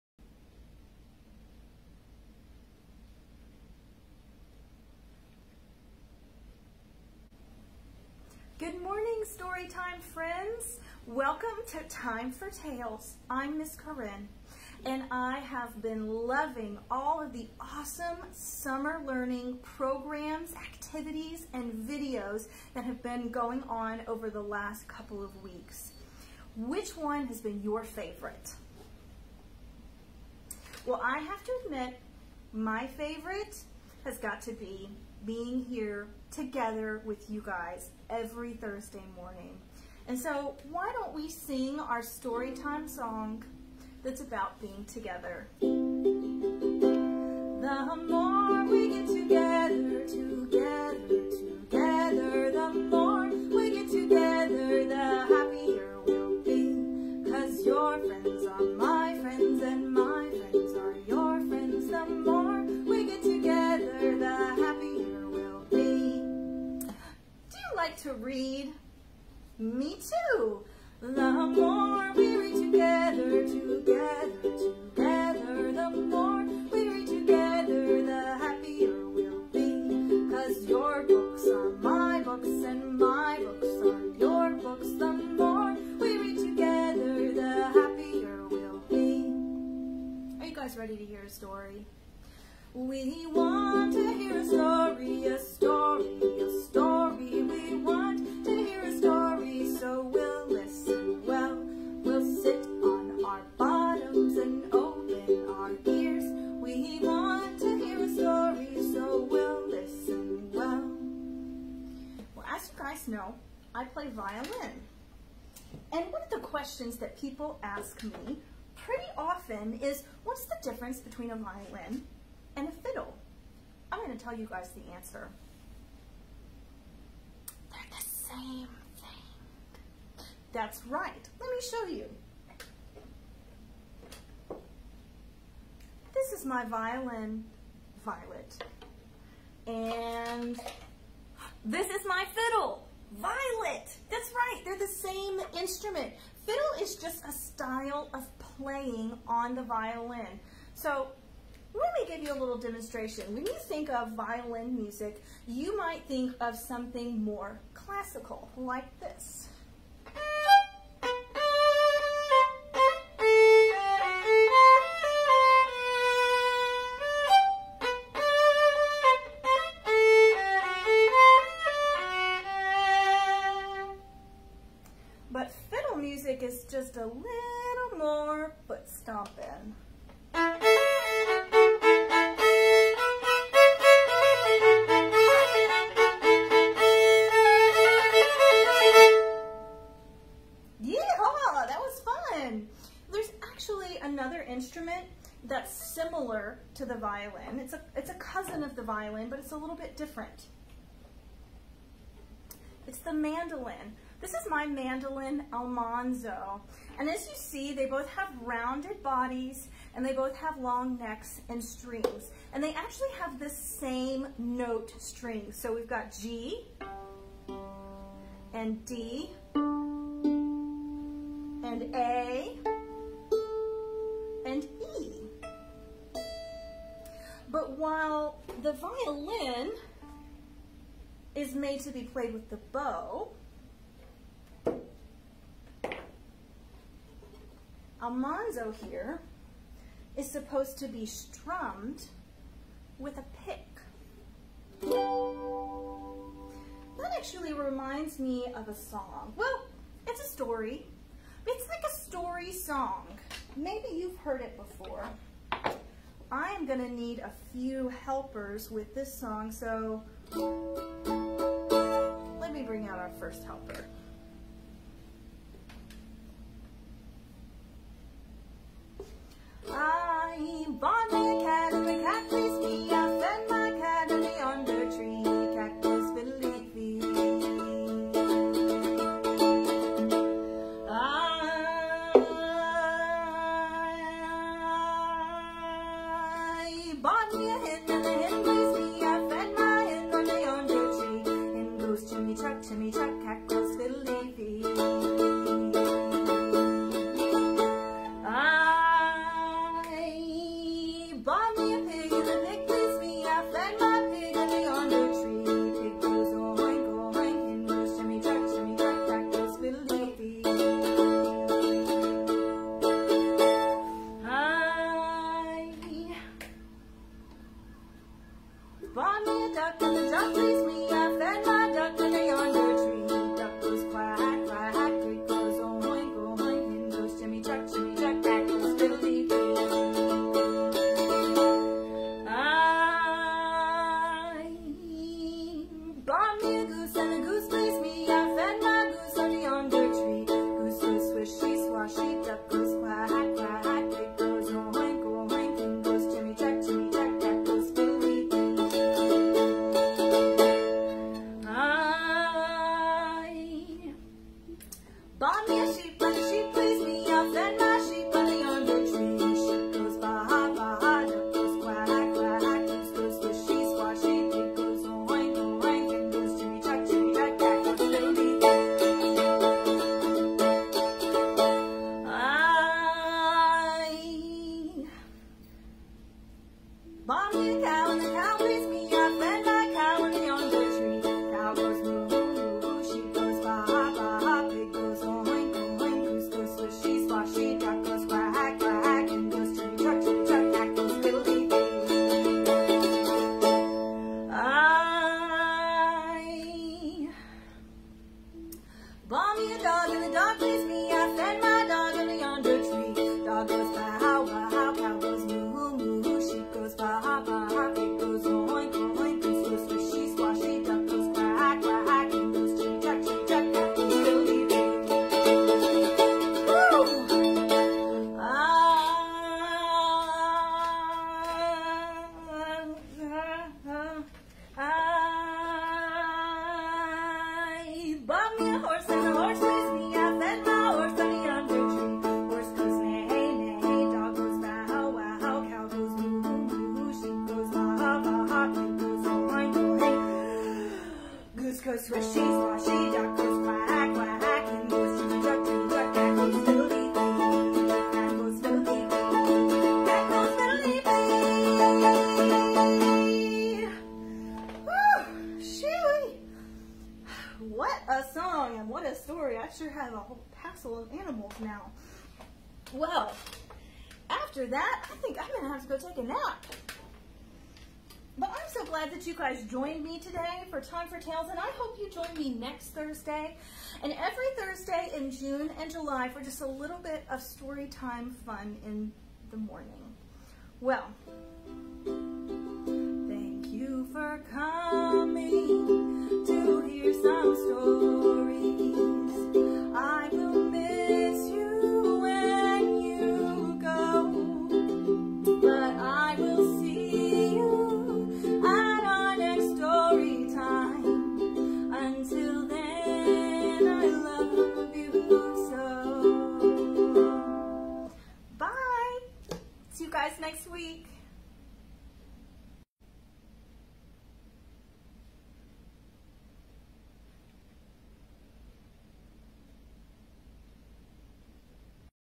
Time for Tales: What’s the Difference Between a Violin and a Fiddle?
Good morning storytime friends! Things get a little silly today as Ms. Corinne gets carried away during a simple explanation. Sing along if you know the song!